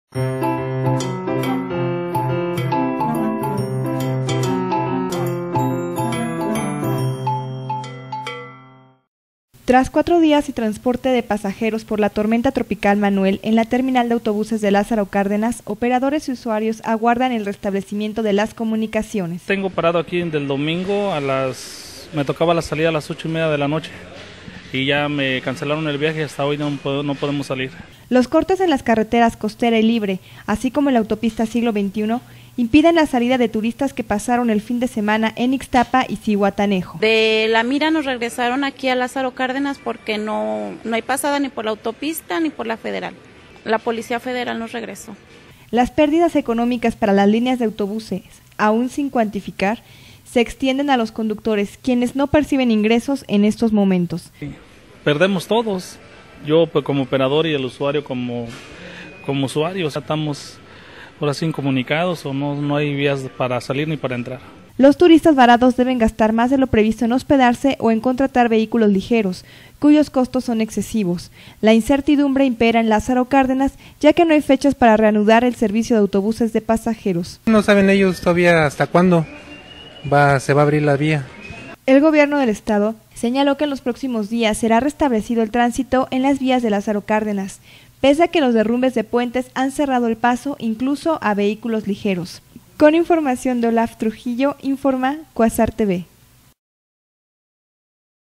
Siguen varados en Terminal de Pasajeros de LC
Tras cuatro días sin transporte de pasajeros por la tormenta tropical Manuel, en la Terminal de Autobuses de Lázaro cárdenas operadores y usuarios aguardan el restablecimiento d elas comunicaciones...